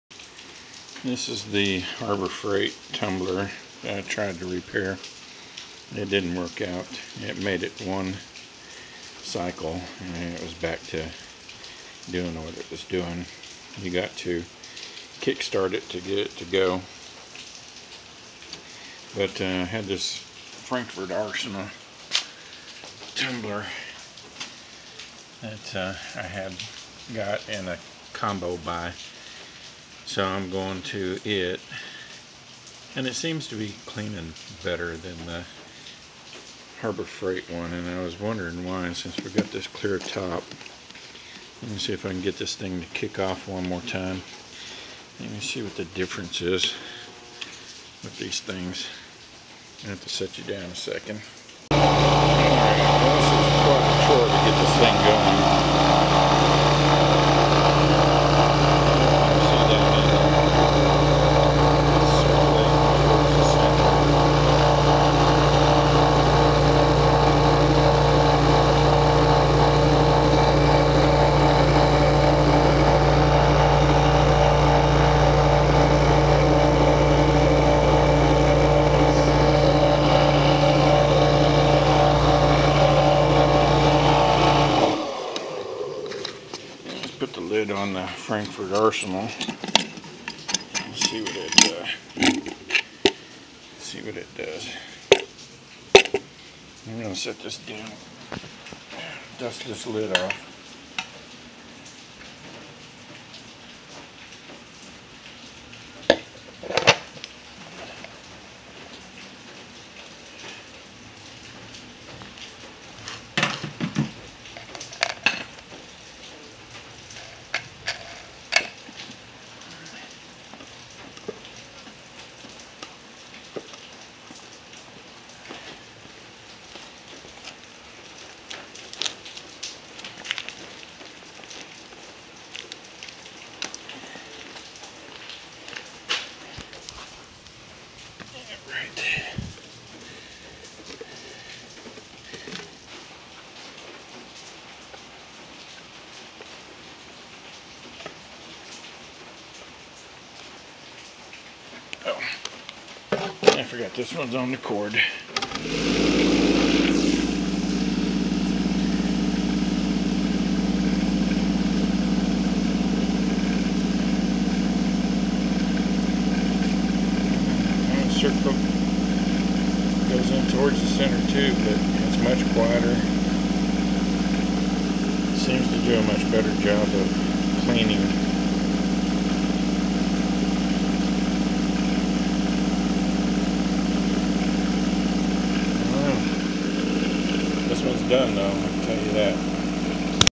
Harbor Freight tumbler repair follow up.
The Harbor Freight tumbler made so much noise you can hardly hear me. It made it one cycle and I liked to have not been able to get it to run to make this video. Used the clear lid to see what or if there was any difference in the shaking of the media, didn't see any, made a hell of a mess though when I had to turn the tumbler over and kick start it.